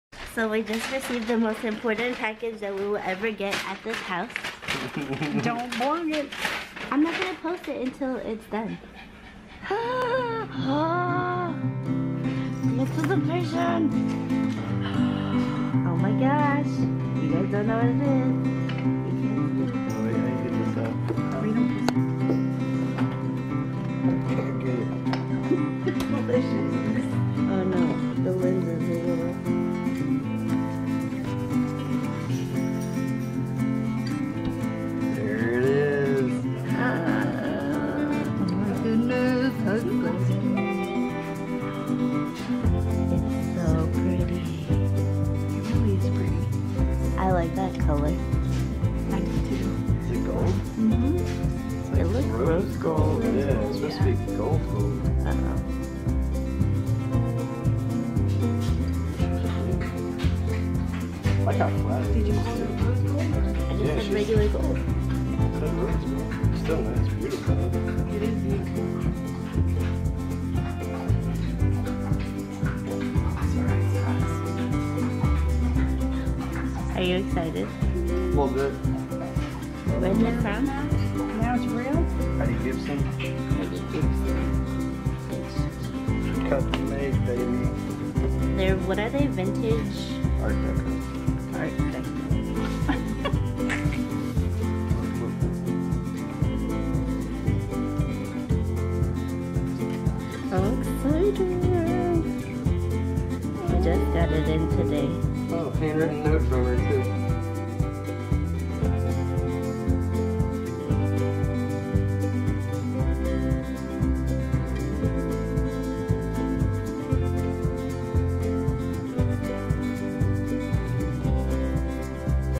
Getting The Ring!!
Just a little something special for Geoff and Cali! We are so excited for Cali to officially become part of our family!